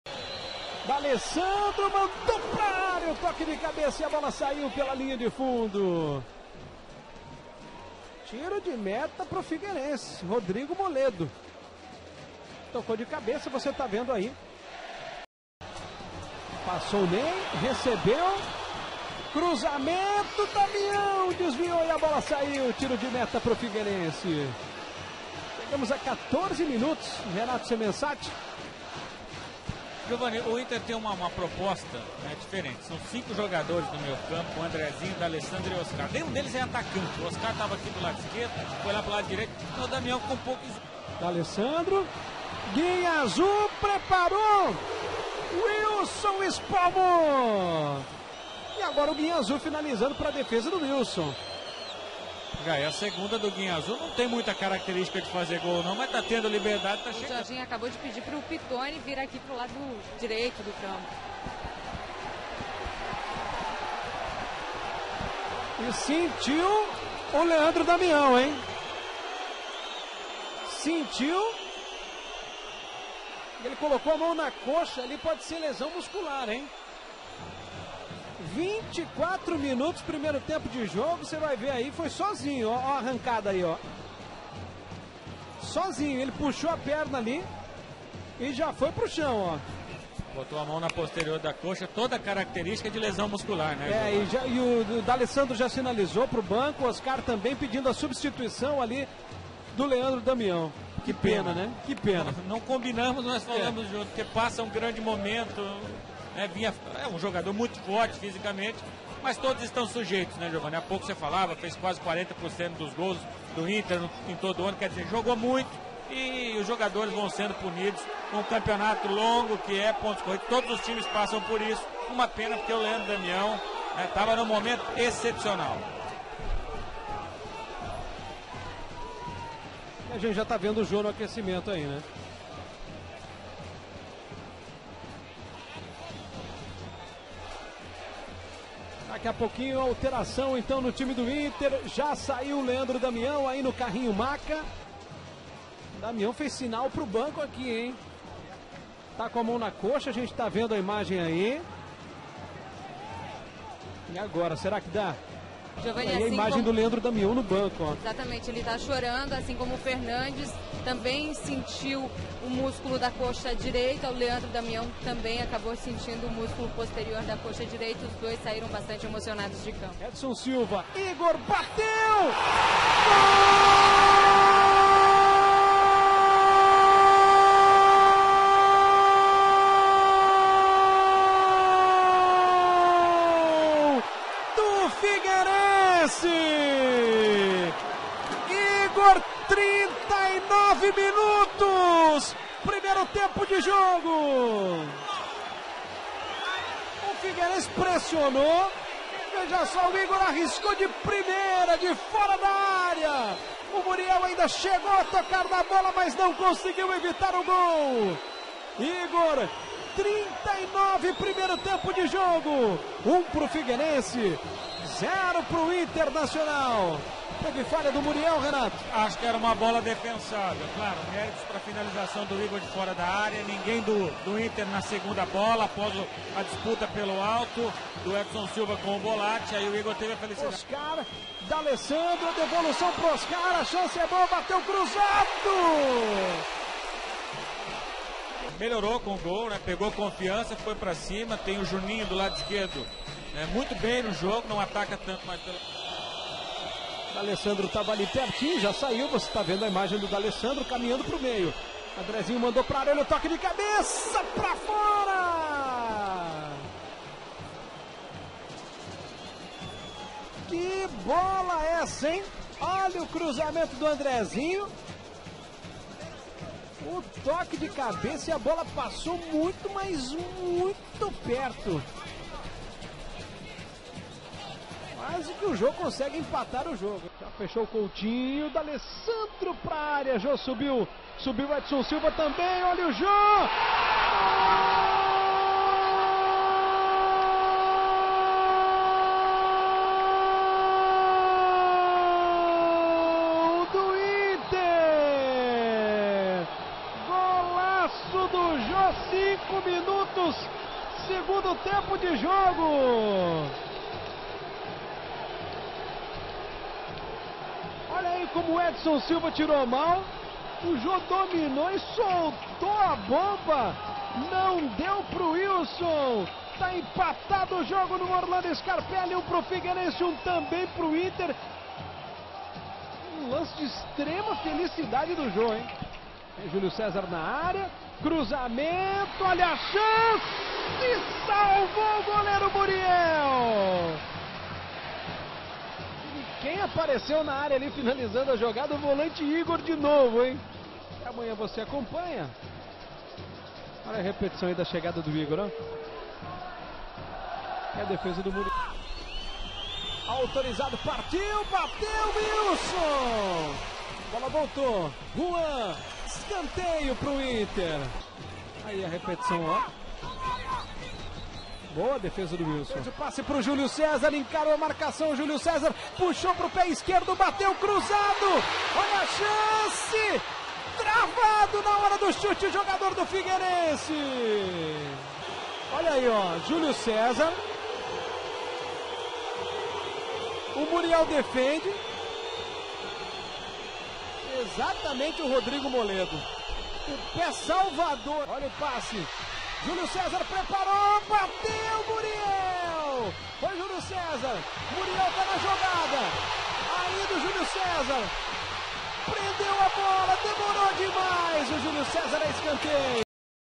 Figueirense 1 x 1 Internacional - Brasileirão 2011
25a rodada - 21/09/2011

Rodrigo Moledo manda de cabeça com perigo.

Nei cruza e Leandro Damião não consegue desviar na direção do gol.

Guiñazu arrisca de fora e Wilson faz boa defesa.

0:58 Leandro Damião está caído no gramado sentindo muitas dores. Ele se machucou sozinho em uma tentativa de arrancada. O artilheiro colorado não não deve ter mais condições de permanecer em campo. Médicos do Inter pedem a substituição de Damião e entra Jô. Leandro Damião chora no banco de reservas do Inter. Nesta quinta tem convocação para Seleção Brasileira.

1 x 0: Após rebote, Ygor arrisca de longe e vence o goleiro Muriel. Arqueiro colorado demora para pular na boa e não alcança.

Oscar tabela com D'Alessandro e chuta rasteiro. A bola desvia na zaga e não vai para o gol.

Andrezinho levanta na área, Rodrigo Moledo desvia de cabeça e bola passa rente ao travessão.

1 x 1: Jô pega rebote dado por Edson Silva e manda uma bomba no ângulo do gol do Figueirense

Após bola cruzada da direita, Ygor entra por trás da zaga e se joga para finalizar. Muriel se estica e faz a defesa.

D'Alessandro cobra colocado e Wilson voa para defender.

Julio Cesar chuta cruzado, Muriel defende e a bola sobra no meio da área. Pittoni chuta travado e não consegue mandar para o gol.

Muriel faz boa defesa em chute de Julio Cesar. No rebote, vai para cima do atacante do Figueira fora da área e tira com os pés.